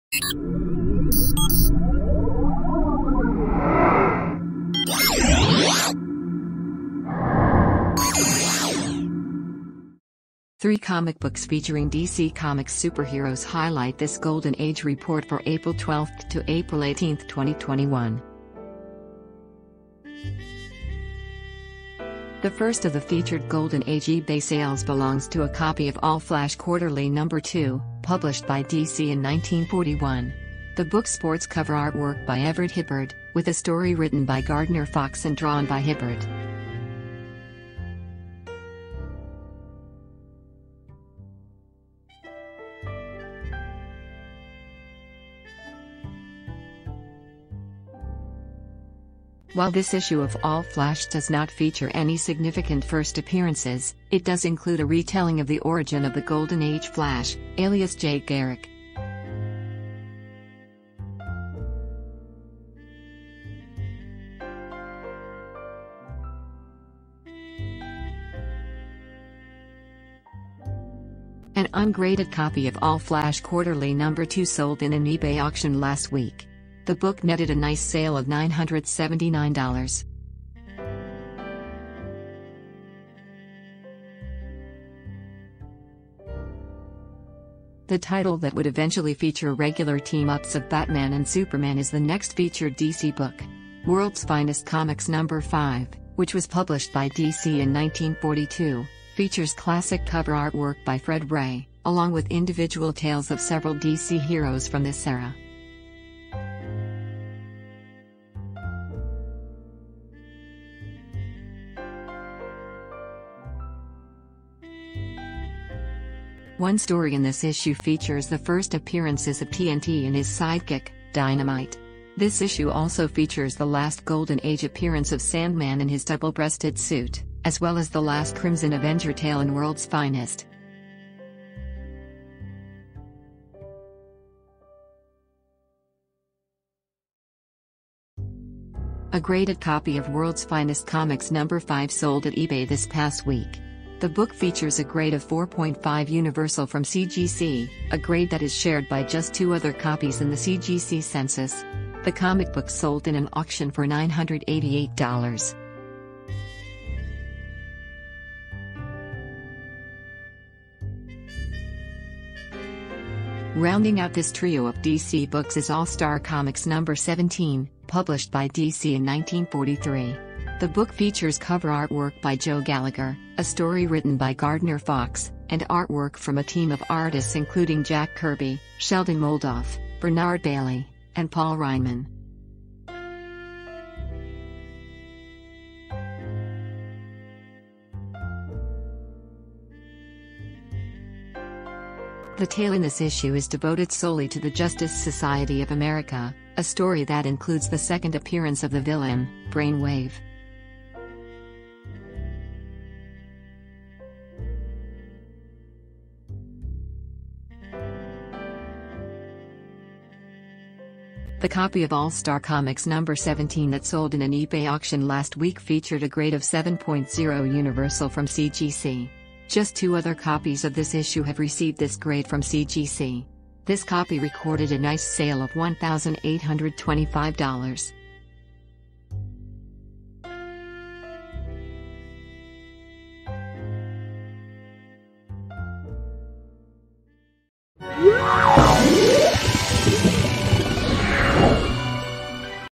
A Trio of WWII-Era Gems from DC Comics Post Nice Sales at eBay
Three DC books from the early 1940's are highlighted in this Golden Age report for April 12-18, 2021. Take a gander at this CBC video production for further details.